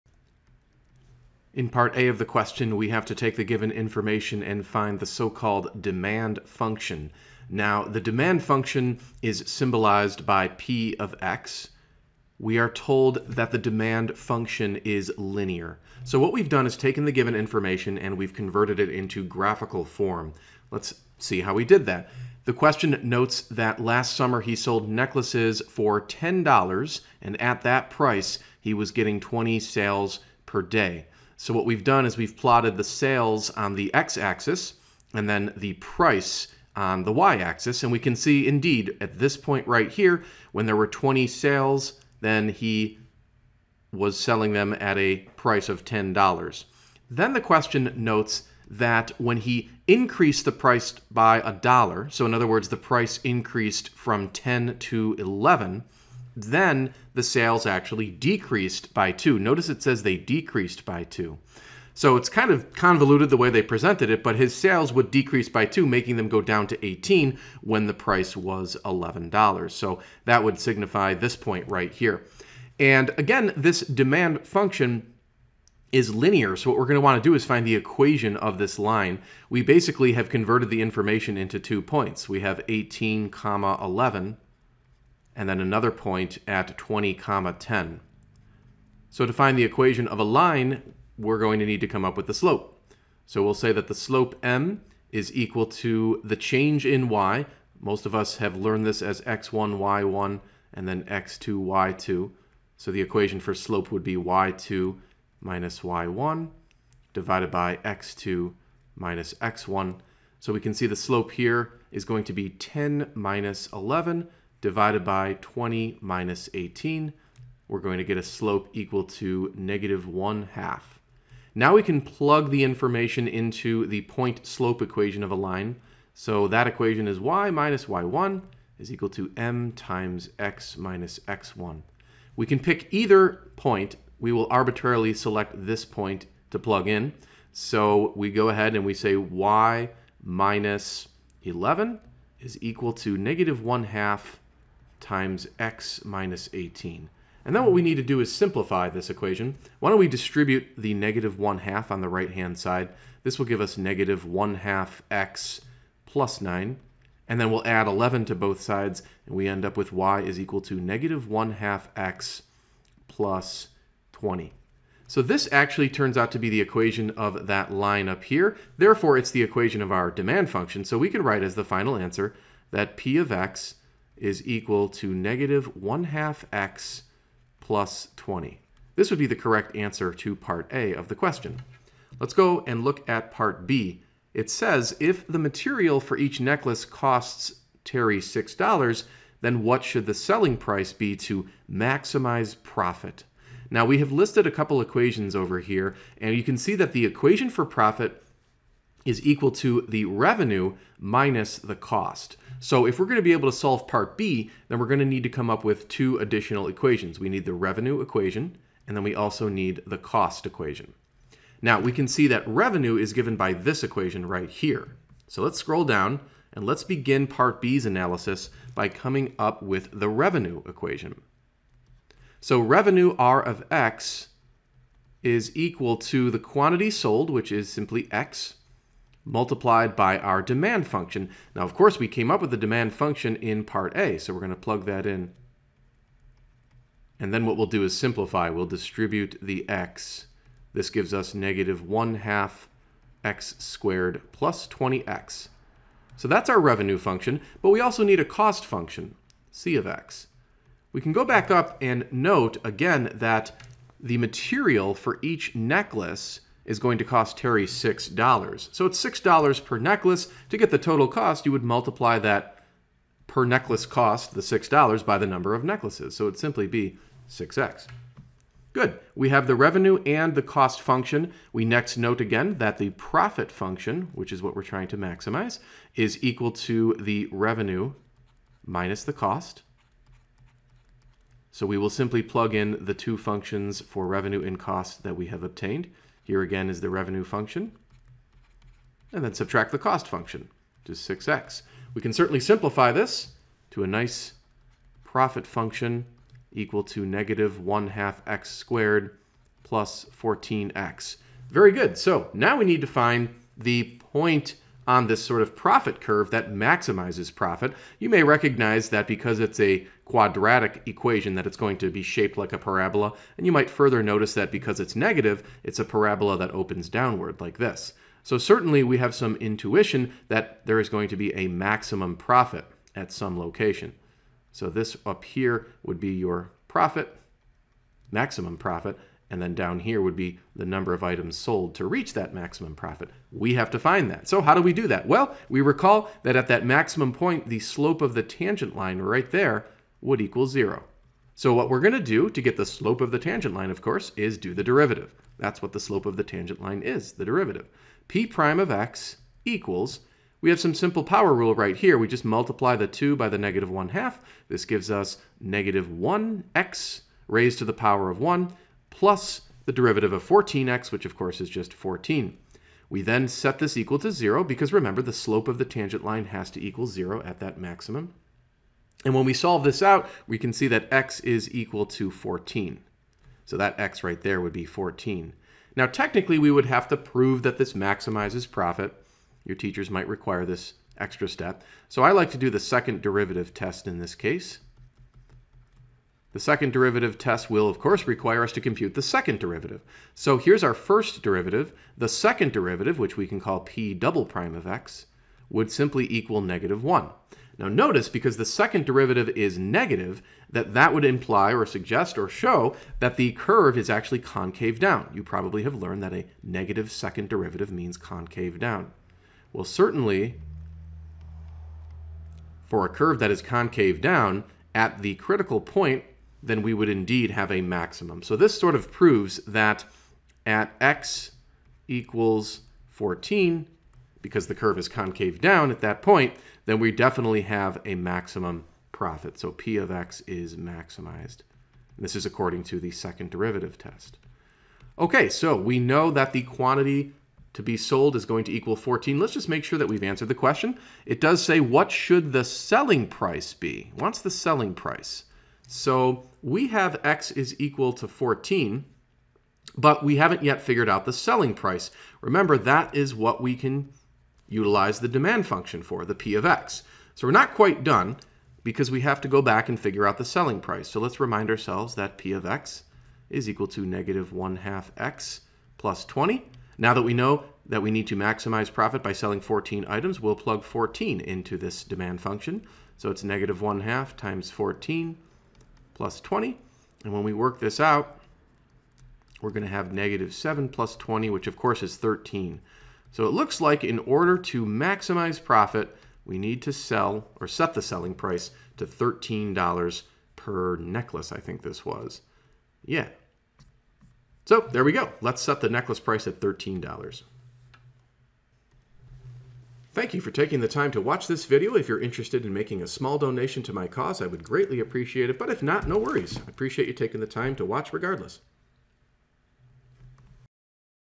During the summer months terry makes and sells
During the summer months Terry makes and sells necklaces on the beach. Last summer he sold the necklaces for $10 each and his sales averaged 20 per day. When he increased the price by $1, he found that the average decreased by two sales per day. (a) Find the demand function, assuming that it is linear. (b) If the material for each necklace costs Terry $6, what should the selling price be to maximize his profit?